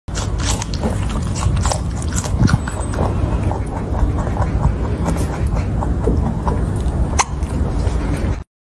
Lovely Bunny eats a pomegranate
Funny bunny is eating a pomegranate